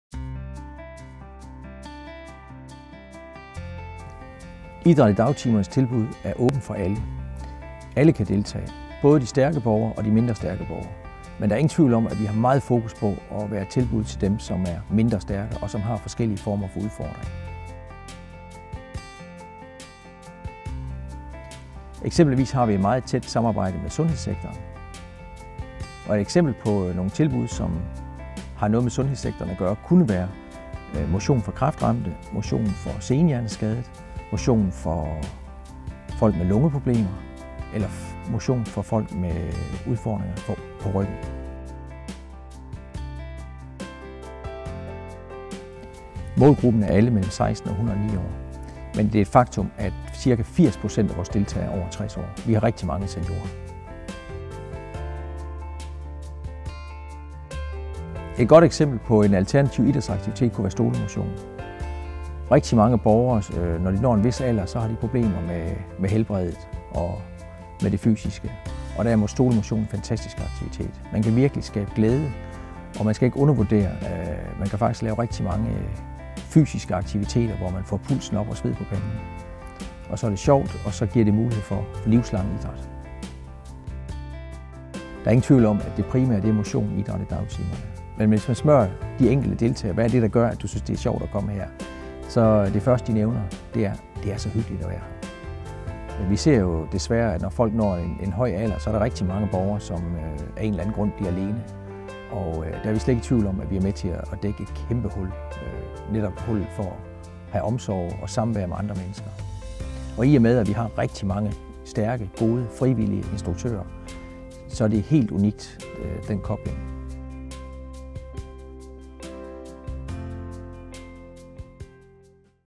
Idræt i dagtimerne - Præsentations film m. tale 2015
Vi er et åbent idrætstilbud, som henvender sig til alle, som har fri i dagtimerne (blandt andet pensionister, førtidspensionister, efterlønsmodtagere, ledige og skifteholdsarbejdere - ja alle som af en eller grund har fri i dagtimerne).